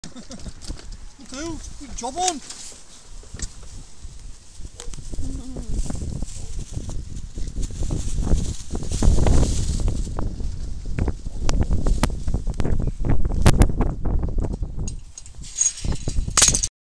10 wheeler articulated 4 wheel drive mobility scooter 4x4x10
The trailer has independant power if needed. (and steering)
Infact you could just drive the trailer if your not bothered about stopping lol.
Is it still legal to use?